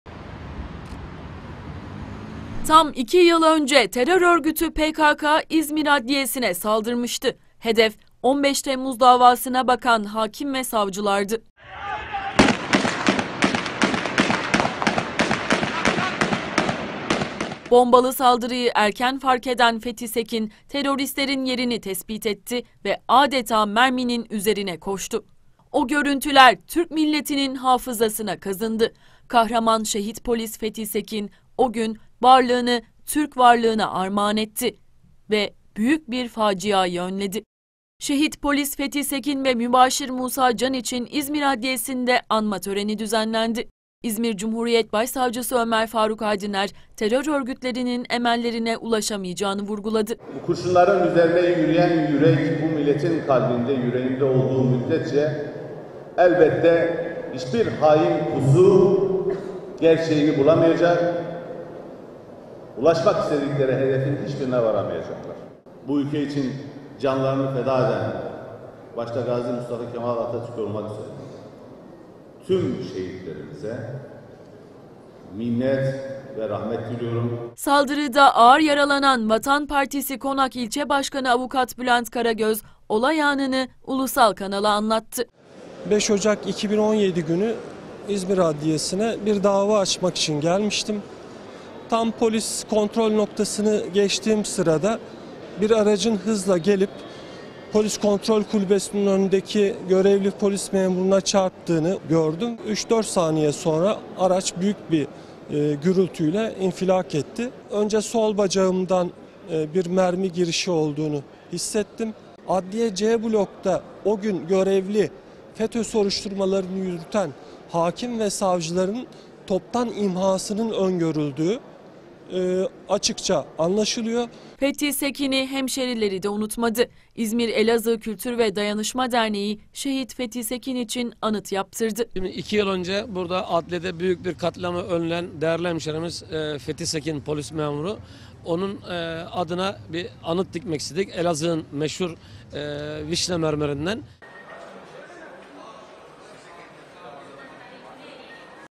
İzmir şehitlerini andı
İzmir Adliyesi’nde iki yıl önce bölücü terör örgütü PKK tarafından bombalı saldırısı düzenlenmişti. Bir kahraman öne çıktı ve varlığını Türk varlığına armağan etti. İşte o kahraman, Şehit Polis Fethi Sekin bugün İzmir Adliyesi’nde anıldı.